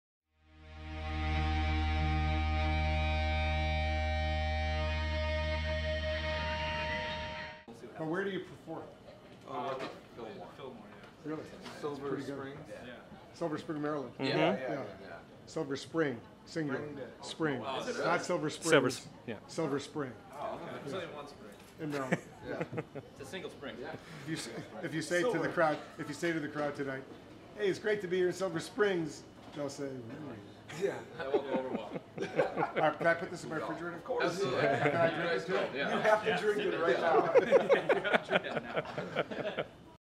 CNN's Wolf Blitzer Meets Every Time I Die
Buffalo metal/punk music outfit, Every Time I Die, drop by CNN in Washington, D.C. to take a tour and meet Wolf Blitzer.

*Note: In all fairness to the band, many people call it "Silver Springs".